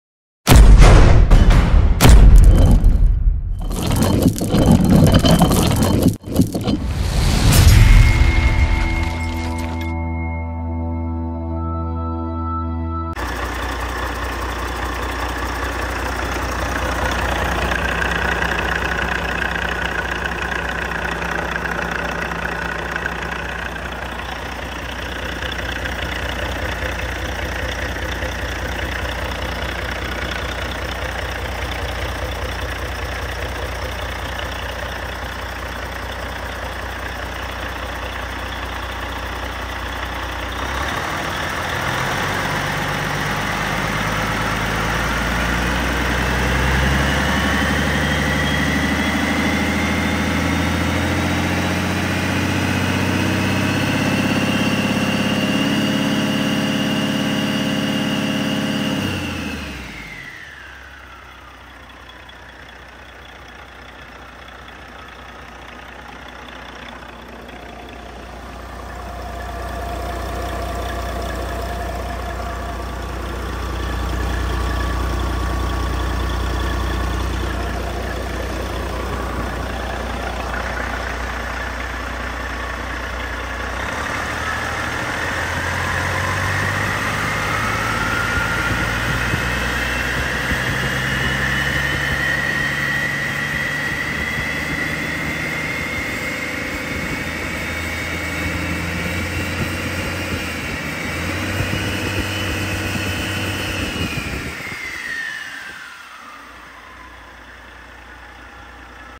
(P-68509) 2005 CATERPILLAR C13 ENGINE ASSEMBLY
(Used) 2005 Caterpillar C13, ENGINE ASSEMBLY- Serial Number: KCB26471, Displacement: 12.5L, Horsepower: 410, For more information about this item, give us a call. We ship Worldwide! (CRG HEAVY TRUCK PARTS) 929 W PULASKI HWY, ELKTON, MD 21921